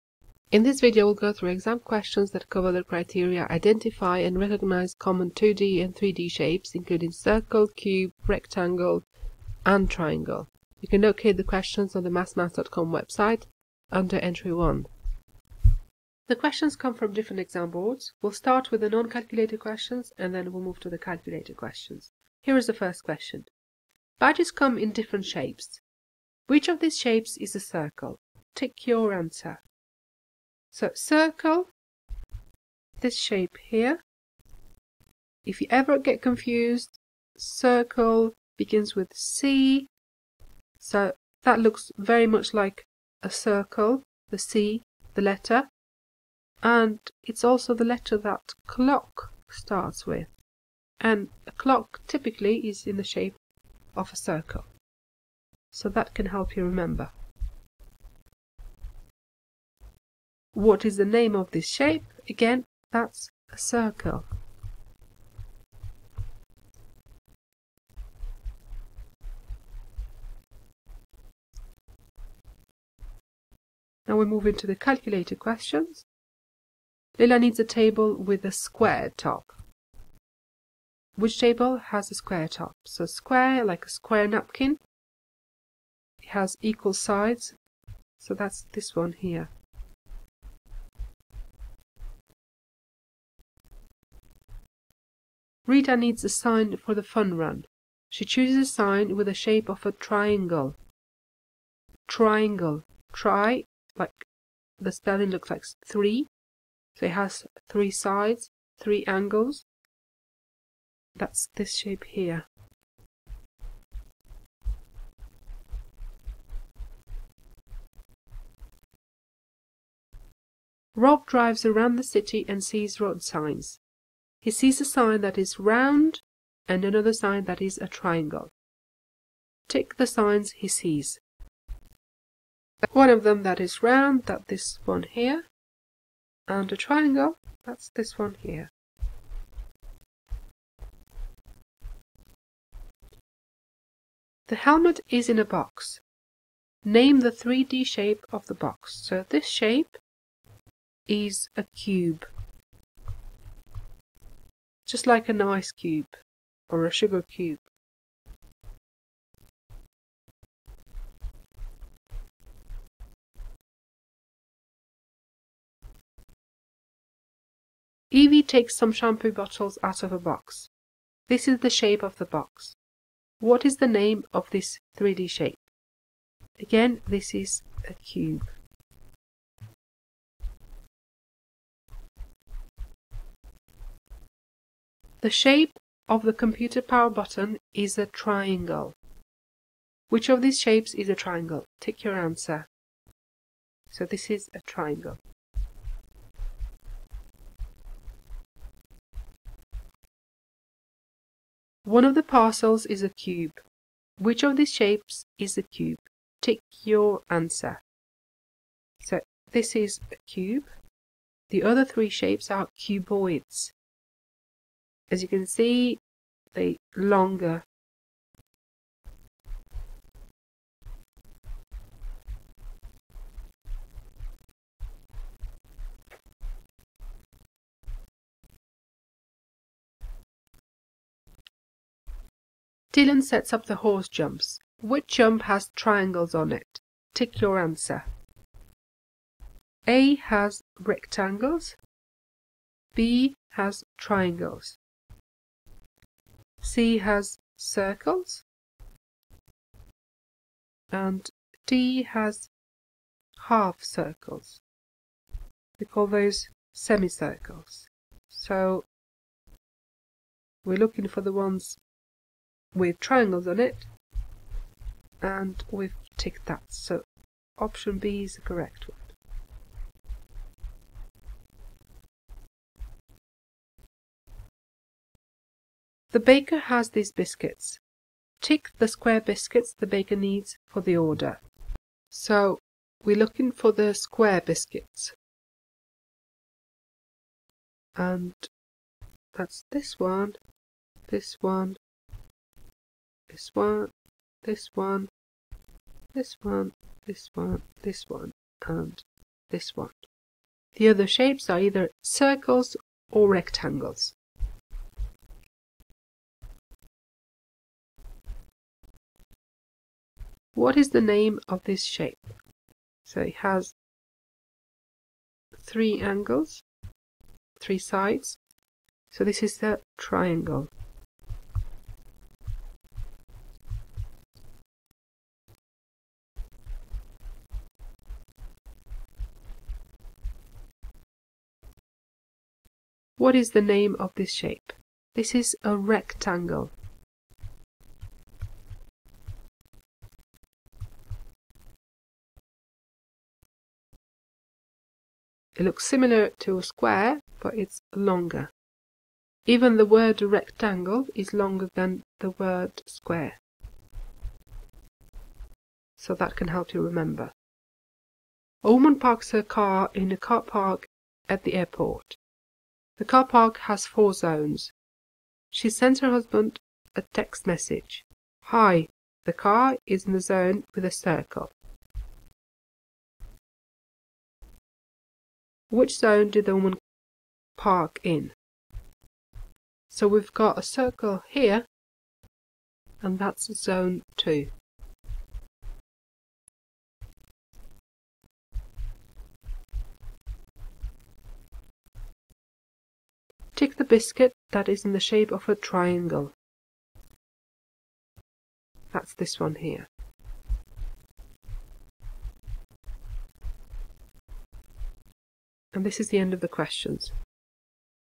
Shapes Exam Questions. Revise for your Entry 1 Functional Skills Maths Exam. 🎯✅️⏰️
Covers: E1.9. Identify and recognise common 2-D and 3-D shapes including circle, cube, rectangle (incl. square) and triangle